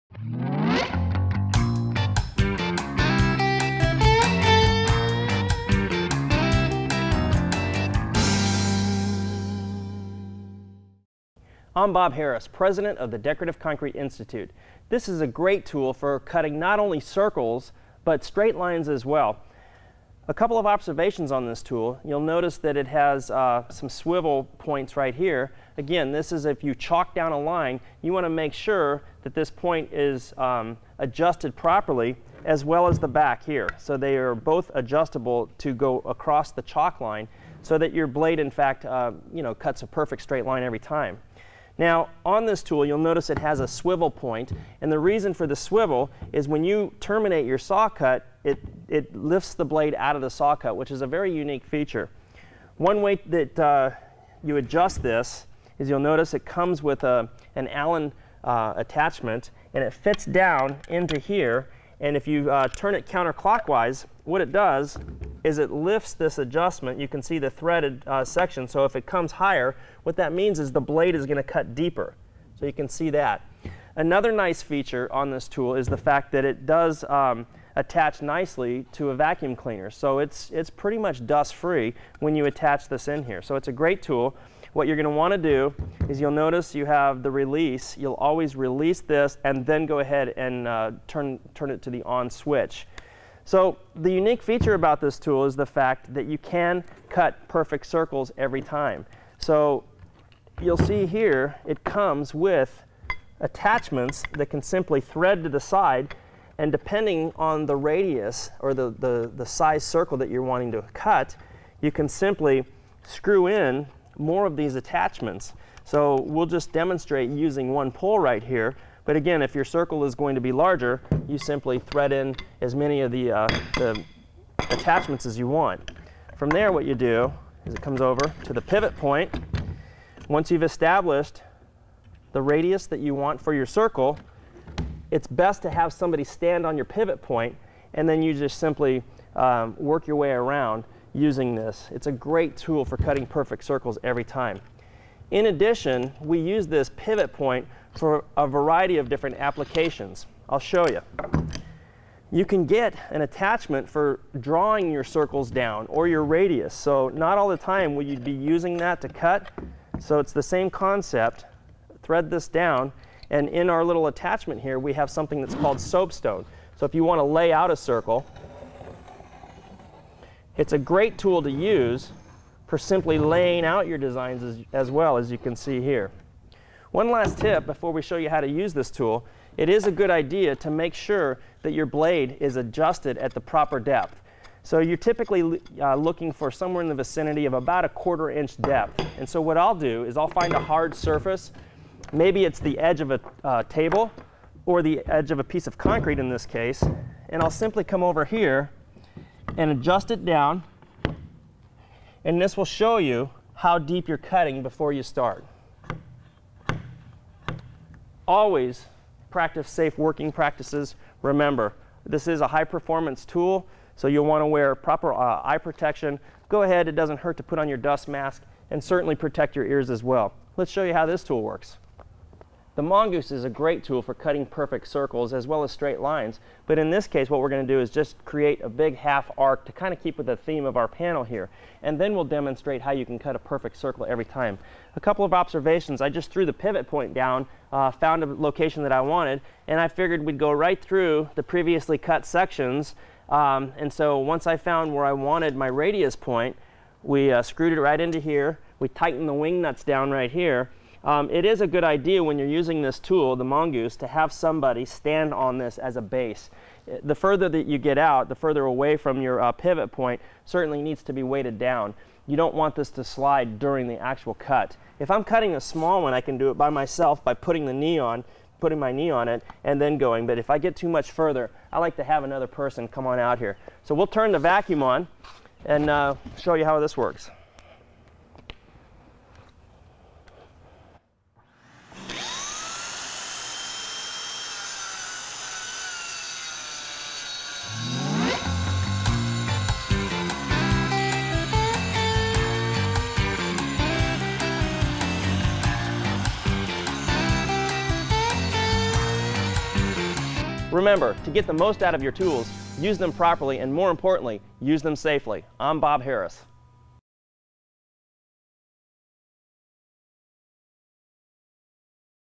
Scored Concrete Using a Mongoose Saw & Blades Video—ConcreteNetwork.com
Mongoose saws and sawing blades. Tips on using this machine to cut intricate designs in concrete surfaces before coloring. The Concrete Network presents Tools of the Trade with Bob Harris.